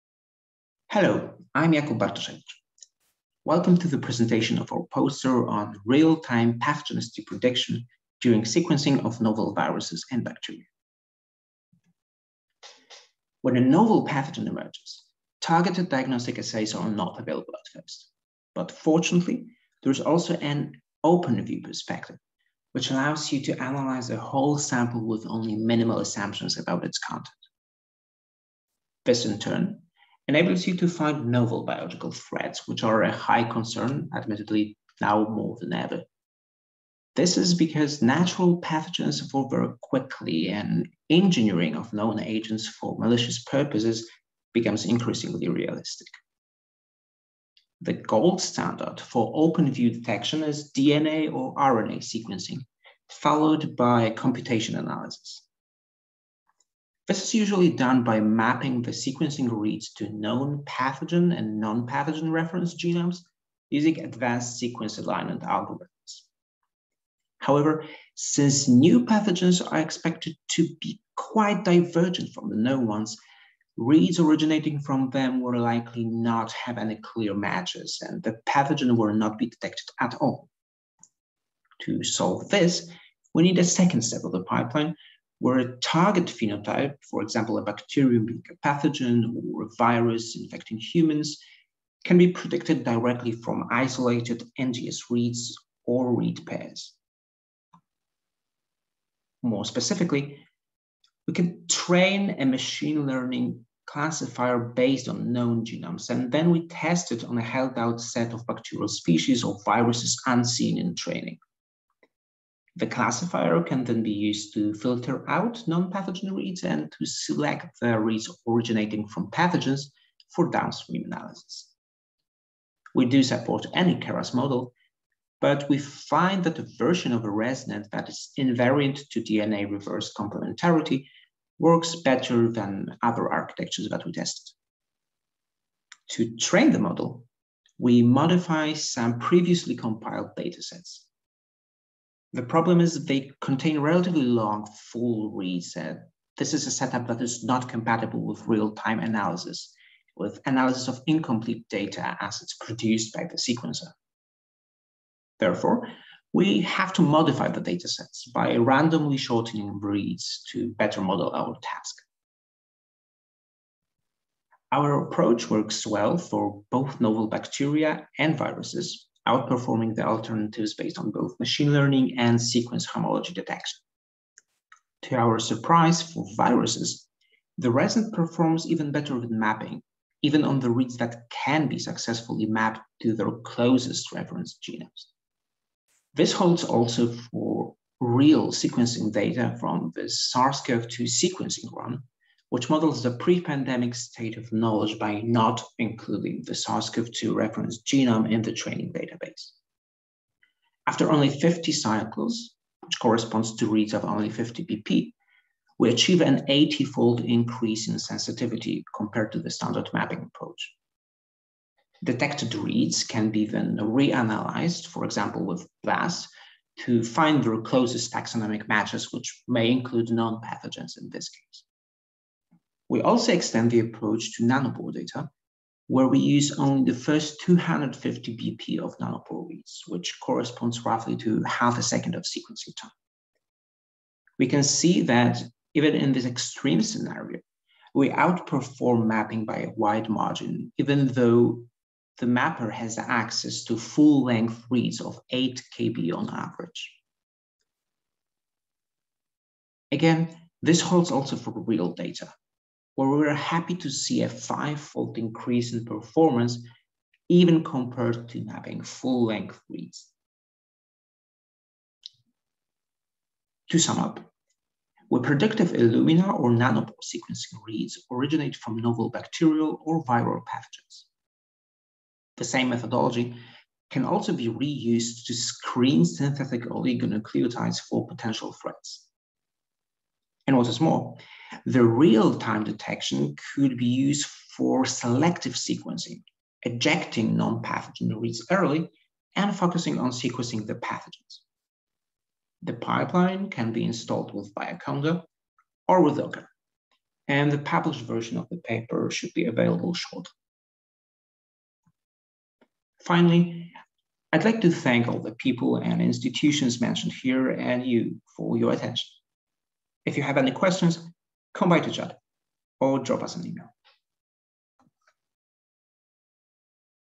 Real-time pathogenicity prediction during... - Jakub Bartoszewicz - MLCSB - Poster - ISMB/ECCB 2021
Real-time pathogenicity prediction during sequencing of novel viruses and bacteria - Jakub Bartoszewicz - MLCSB - Poster - ISMB/ECCB 2021